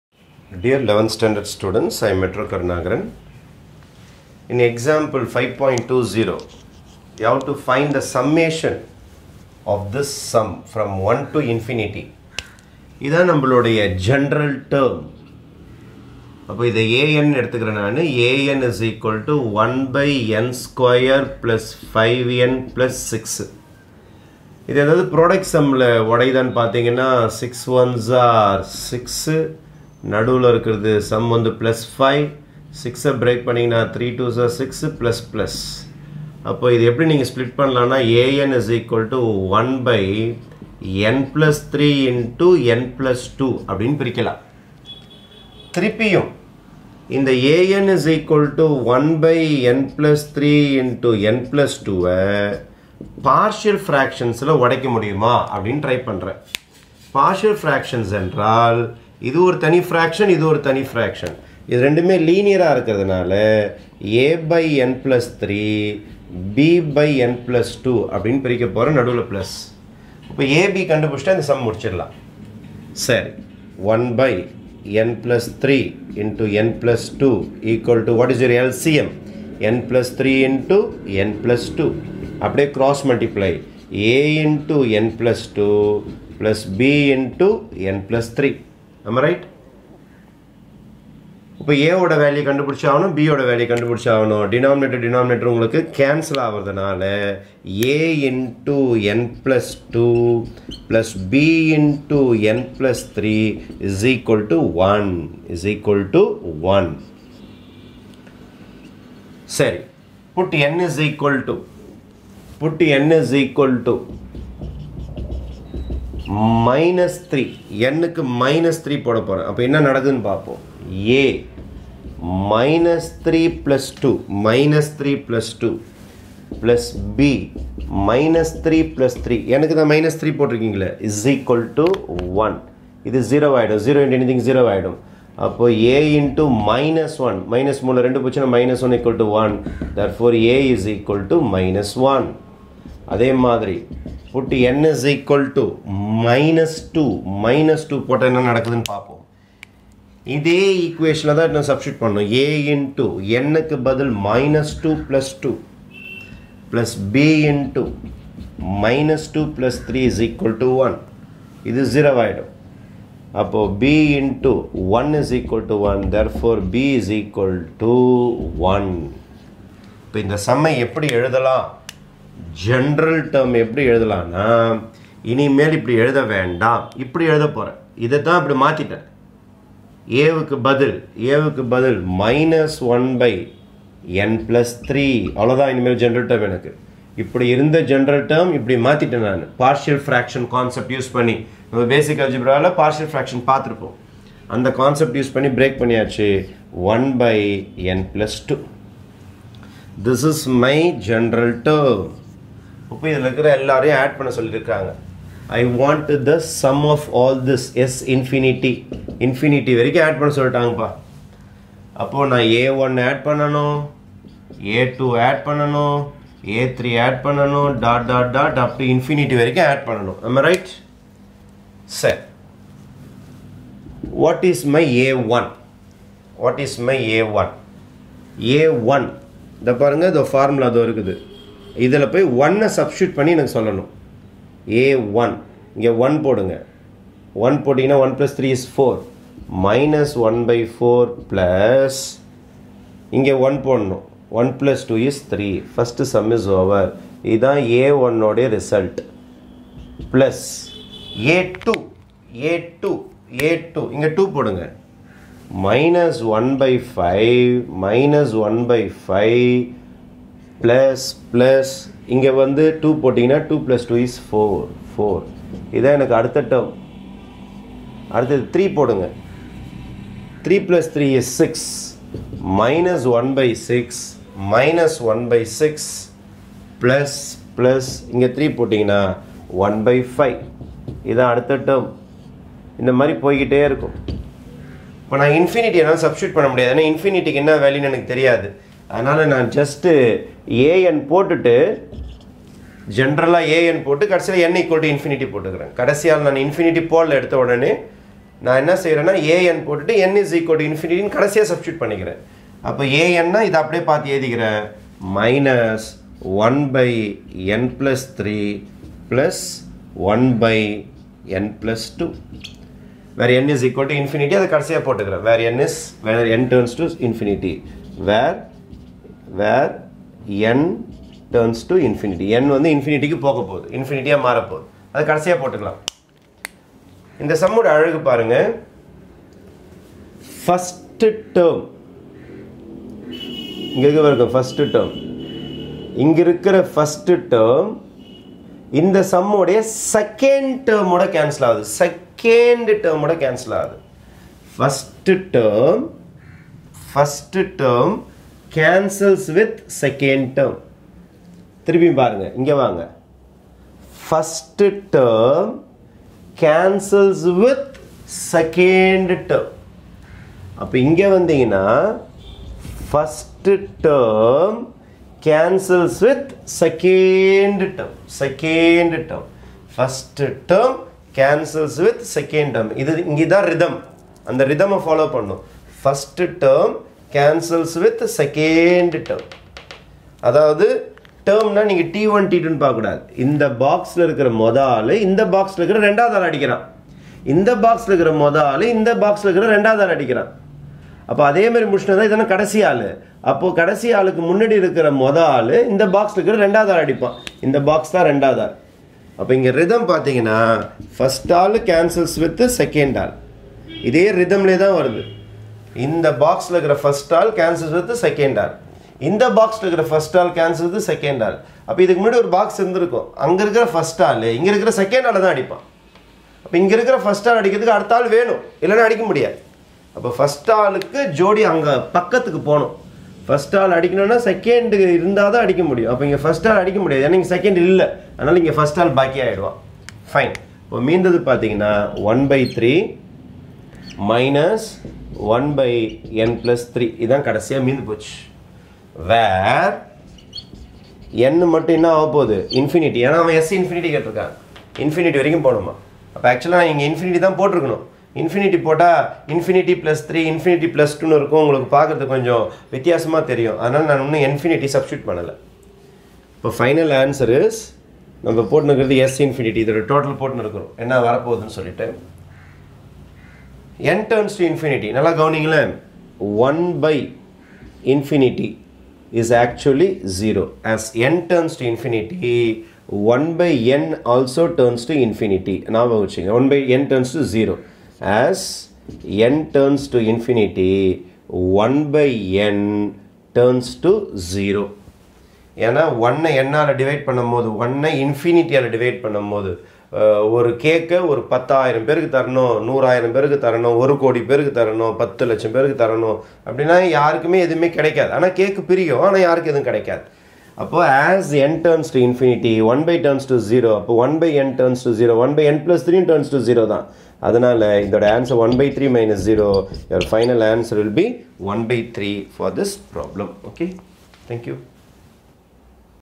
11th Std Maths Example 5.20 Find Sigma n= 1 to infinity (1/n²+5n+6)
11th Std full solution e-book.

CHAPTER 5 BINOMIAL THEOREM SEQUENCE AND SERIES

EXAMPLES